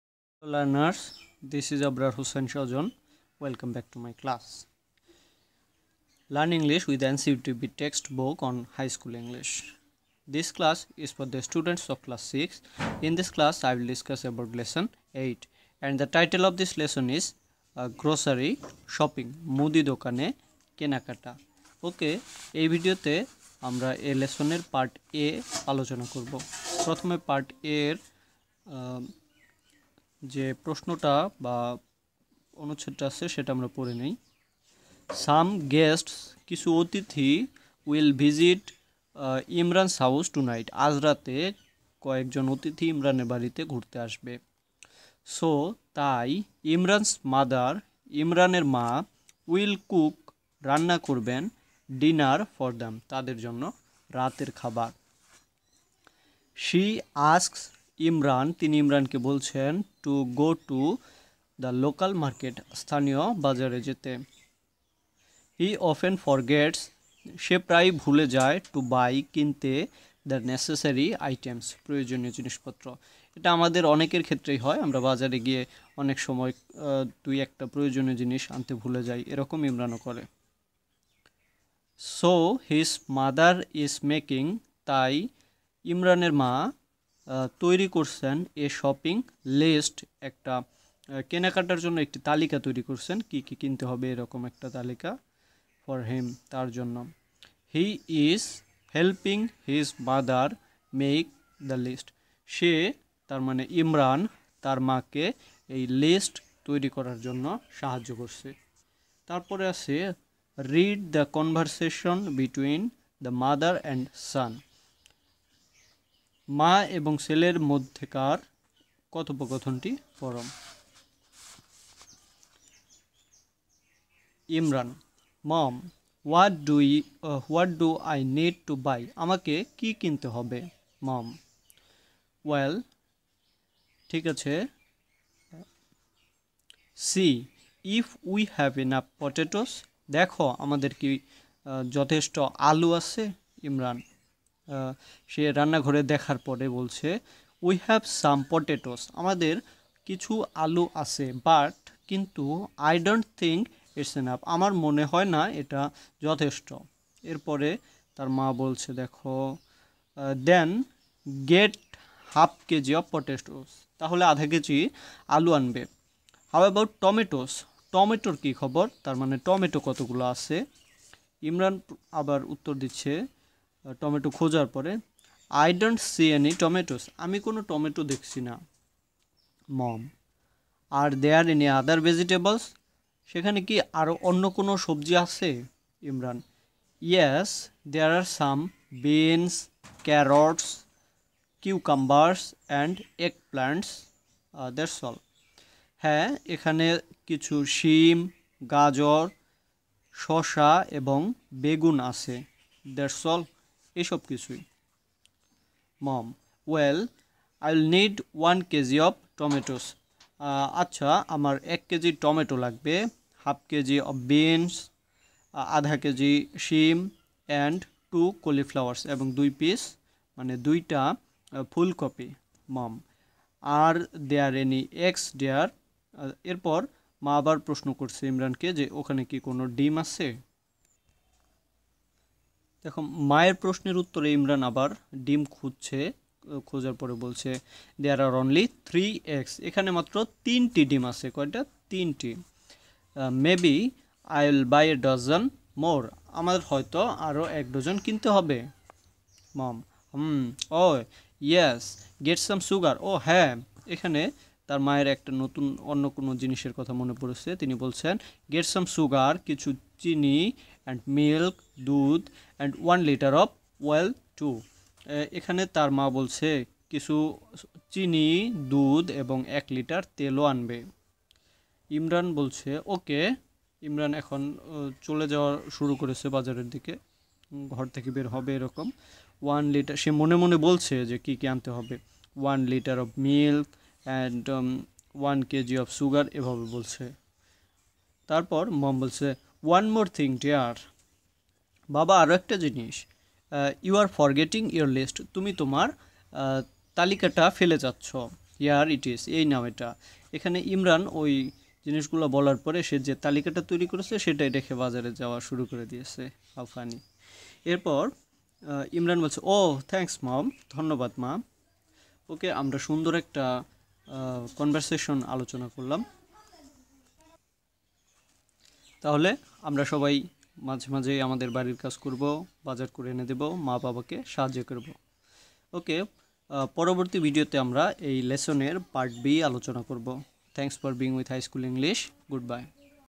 Class 6 Lesson 8, Part A || High School English
This video is for learning English with NCTB text book for the students of class six.